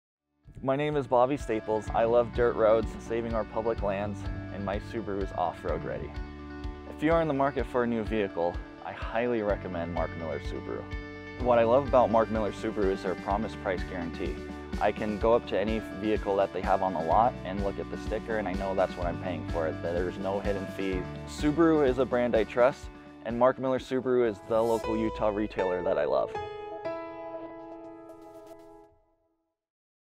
My Subaru Is...Off-road Ready (Sales)
Meet Bobby, a Subaru enthusiast who loves off-roading and using his Outback for outdoor experiences. Bobby has been a member of MtnRoo Utah for many years–a like-minded group of Subaru owners that promote a welcoming and helpful environment within the off-road and adventurous Subaru community.  As part of the Subaru Ambassador program and MtnRoo Utah, Bobby helps organize and participates in outdoor clean-up events, clothing drives for those in need, and social meet-up events for the Subaru community. Bobby and his wife recently had their first child, so his Subaru needs are starting to evolve. Mark Miller Subaru is excited to have Bobby as part of our extended Subaru family and support him as his Subaru needs change.

Adventure awaits for Bobby, watch his full story here.